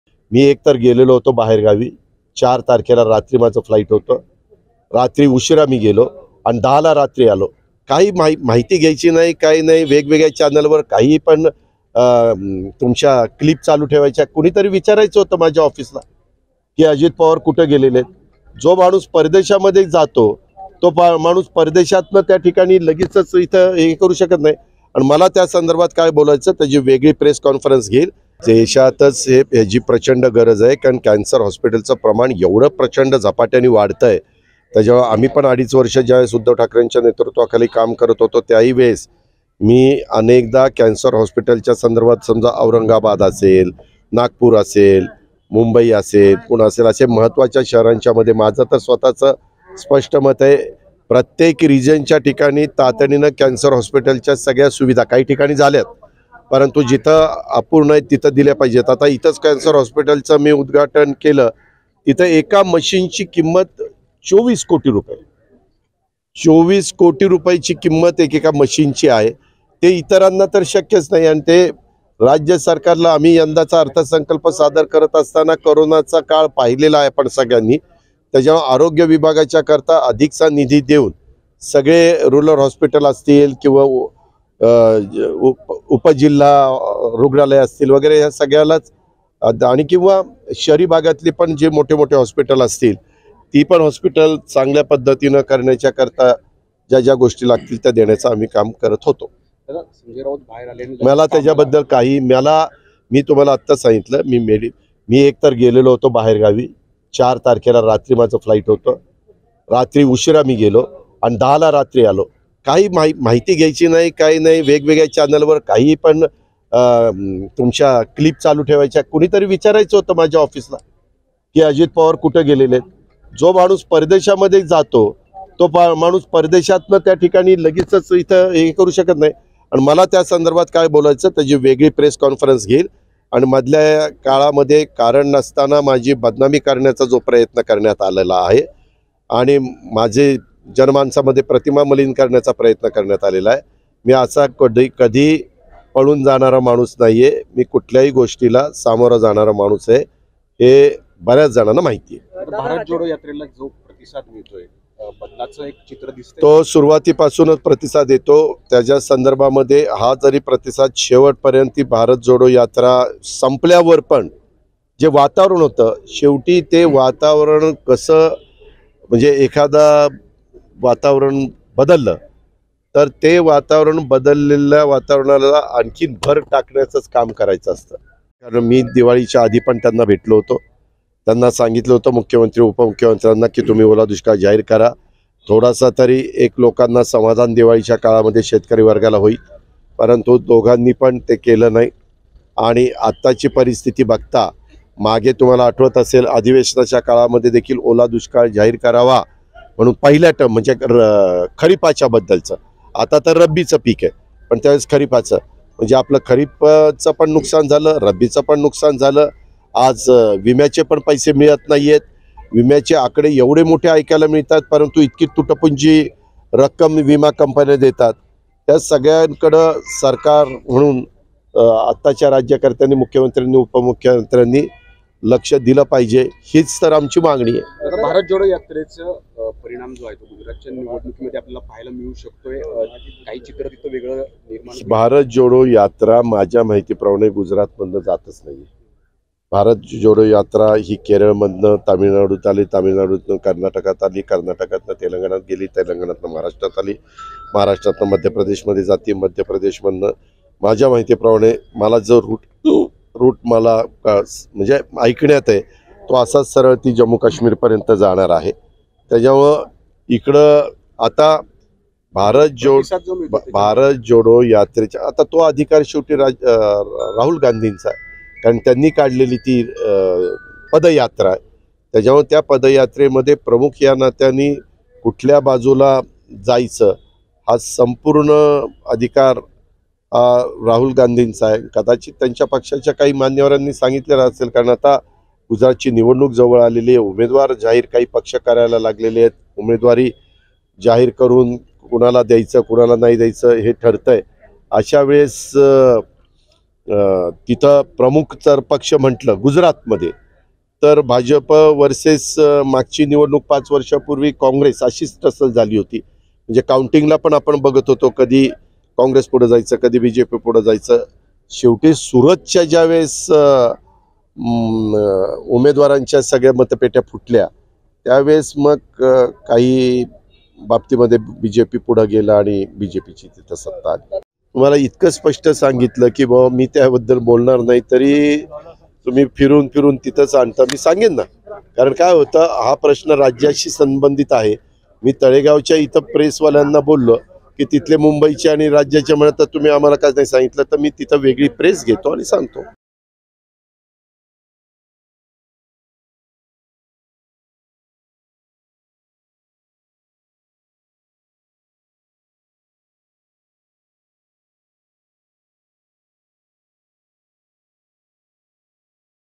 Ajit Pawar : अजित पवार का होते नॉट रिचेबल? त्यांनीच सांगितलं कारण... | LetsUpp Marathi
गेल्या काही दिवसांपासून अजित पवार हे नाराज असल्याच्या चर्चा रंगल्या होत्या. त्यावर आता खुद्द अजित पवारांनी खुलासा केला आहे. अजित पवार नेमकं काय म्हणाले? पाहूयात...